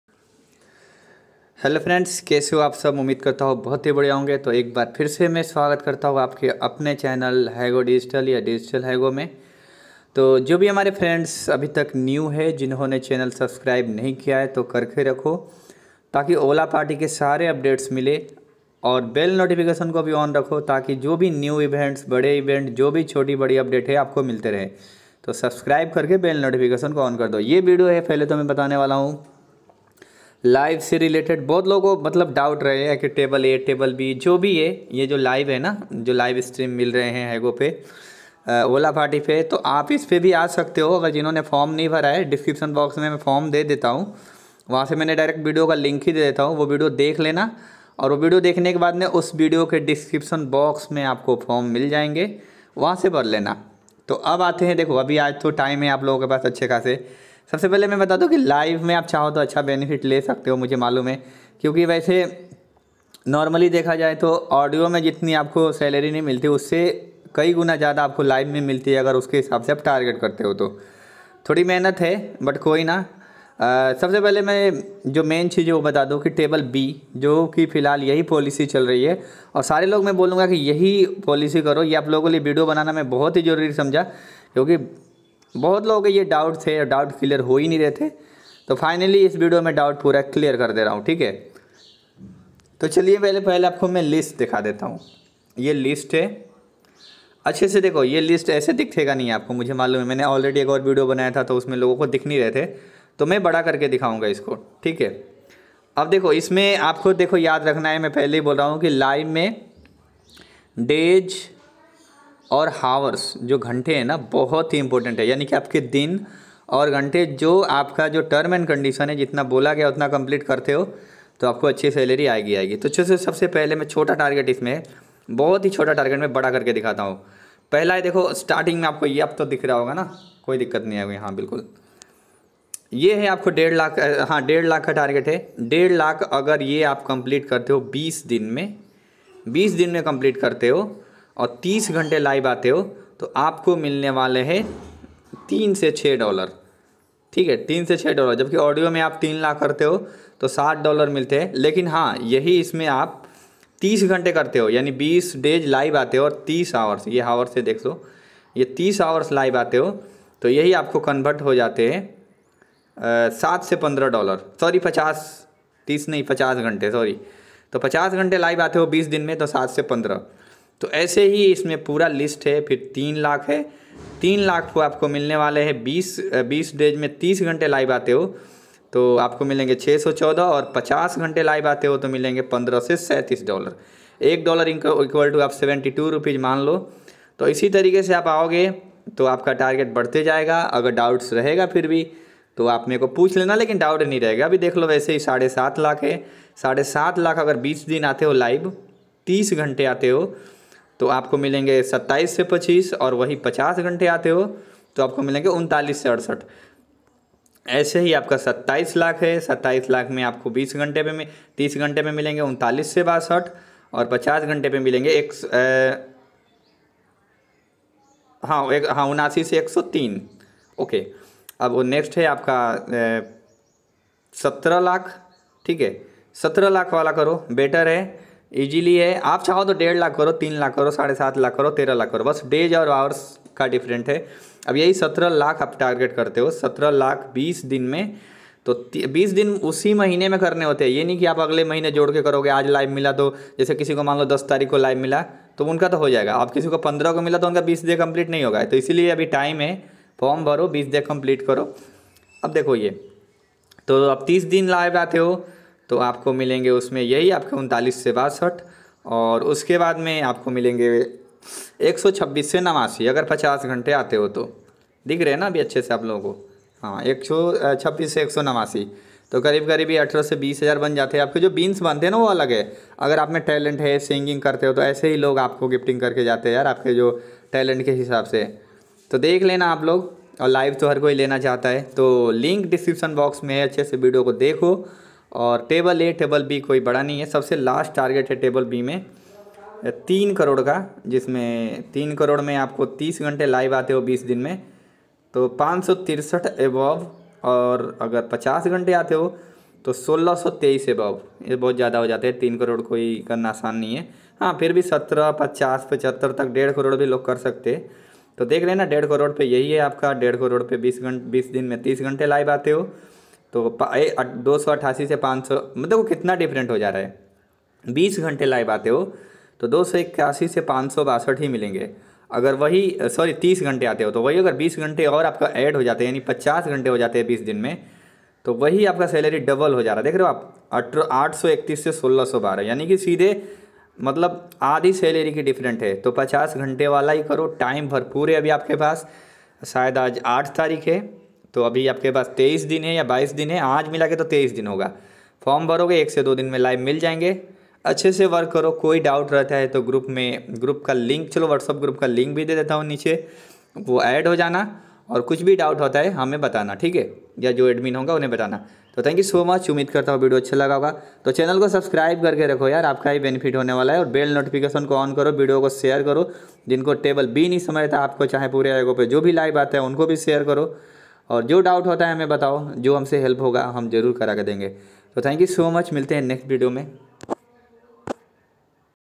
OlaParty | Salary Chart | Table B याहा समझें| In Hindi ||By HD
Hello friends is video me bataya gaya hai ki ap olaParty pe live aate ho to apki salary list kya hai.. Or live kaise ana he uska bhi. Link niche diya gaya he.. So watch this video carefully...!